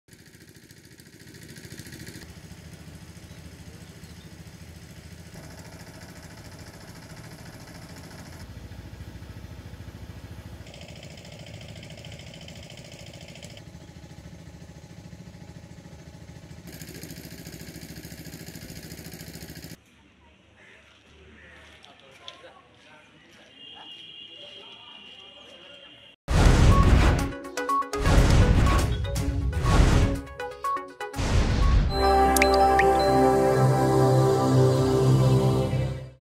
ଟ୍ରକକୁ ପଛରୁ ପିଟିଲା ଟ୍ରକ
ଢେଙ୍କାନାଳ କାମାକ୍ଷାନଗରରେ ଟ୍ରକକୁ ପଛରୁ ପିଟିଲା ଟ୍ରକ...ଦୁର୍ଘଟଣାରେ ଜଣେ ମୃତ...